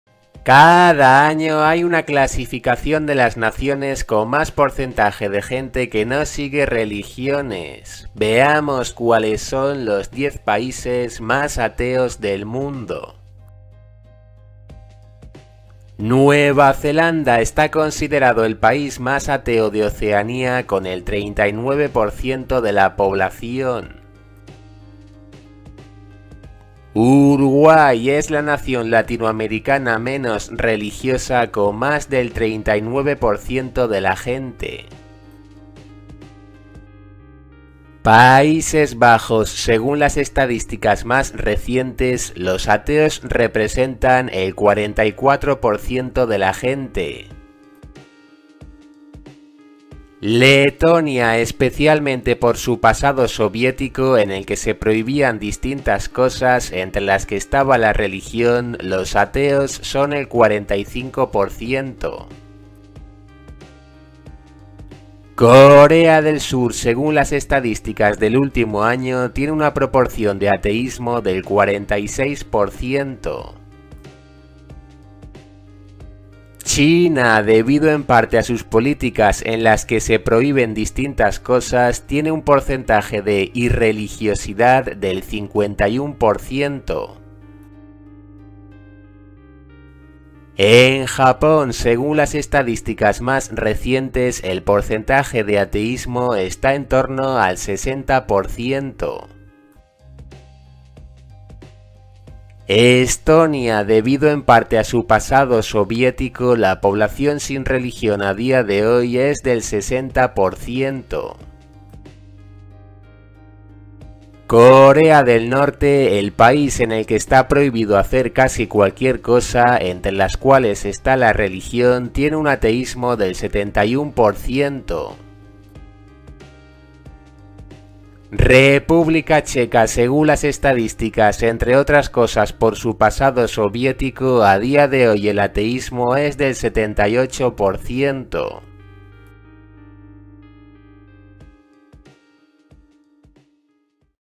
LOS 10 PAÍSES MÁS ATEOS DEL MUNDO (2021)
Los países más ateos de todo el mundo.
Más de 7 años haciendo vídeo diario sin fallar un solo día.